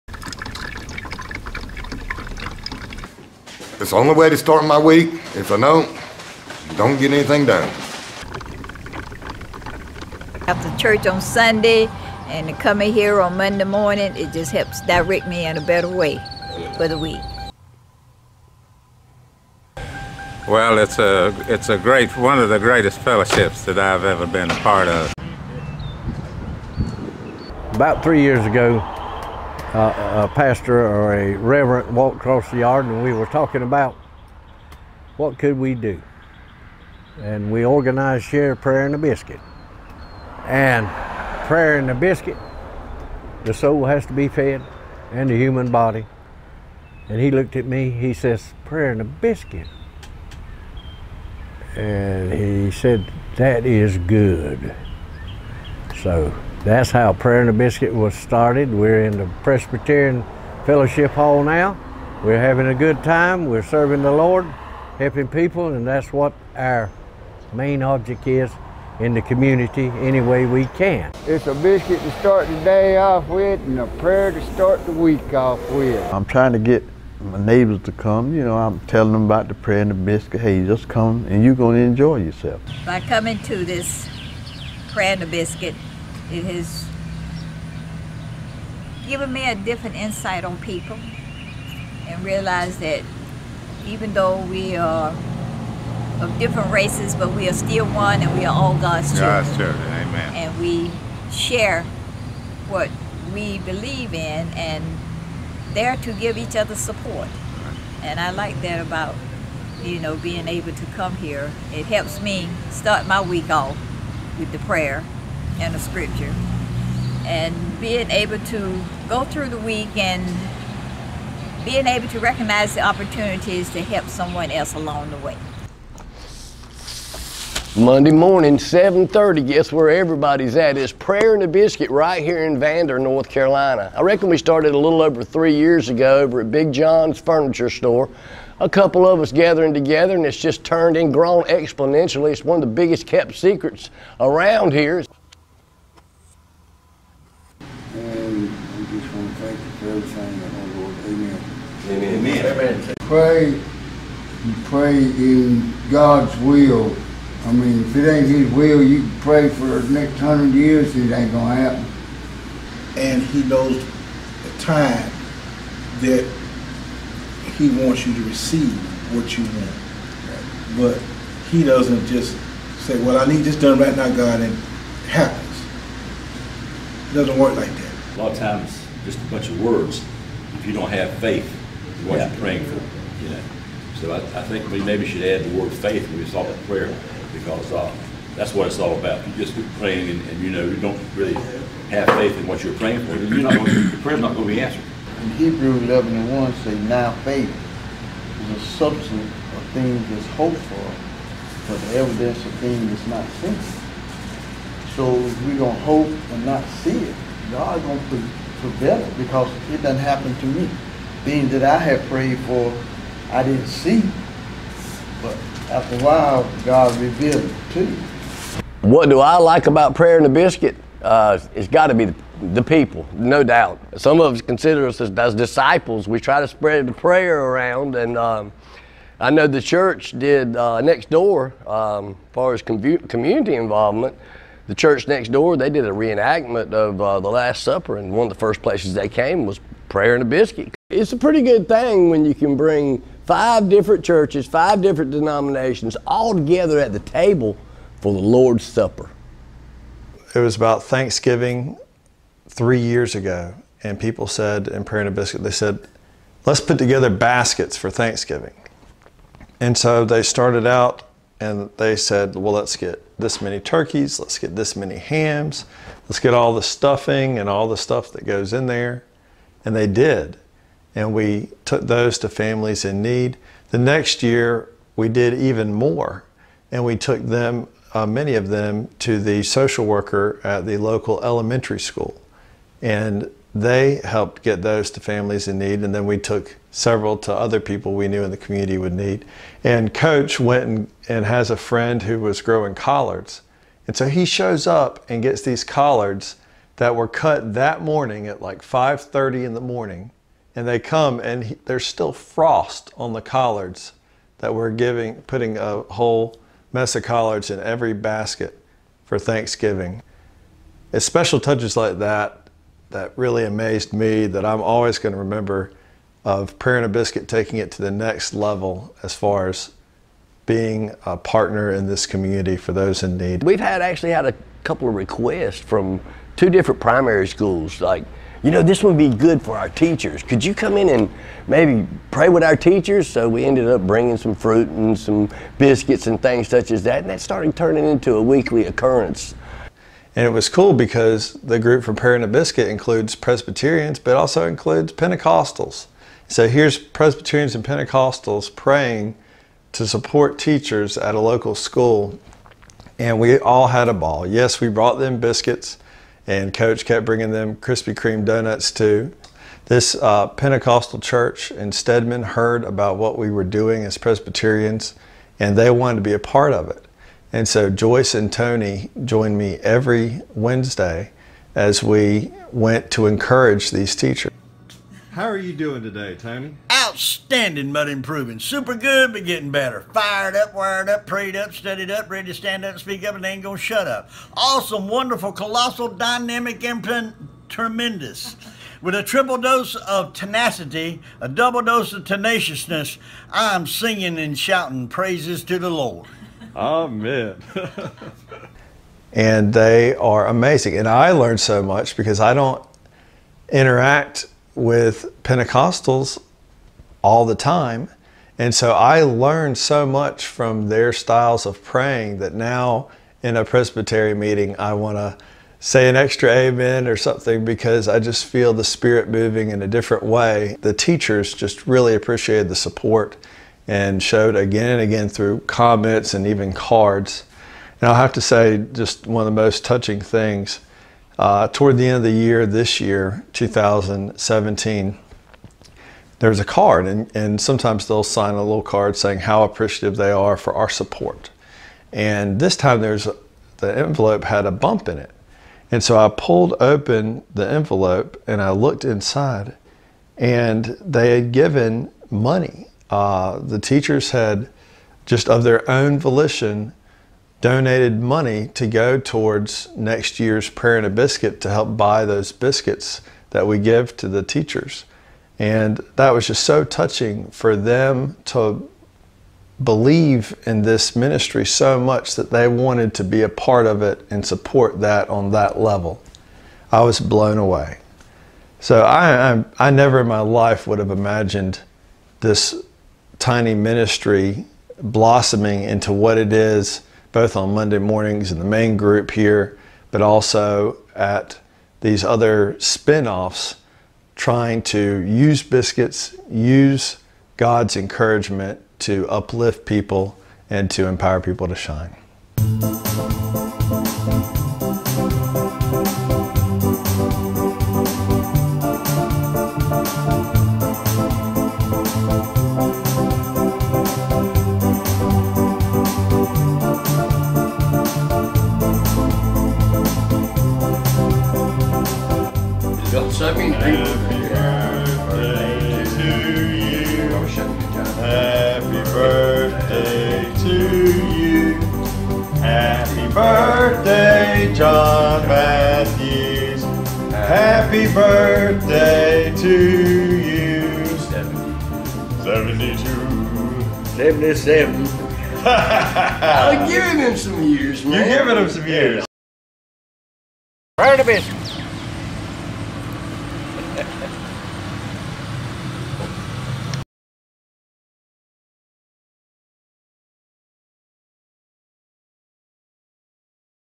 Prayer and a Biscuit
What began as a prayer breakfast grows in community outreach touching the lives of dozens in Vander and Stedman, NC on the eastern outskirts of Fayetteville. Prayer and a Biscuit meets in the fellowship hall of Sunnyside Presbyterian Church This video was filmed by Evan Harrison as part of the sacred stories project . Watch in 4k!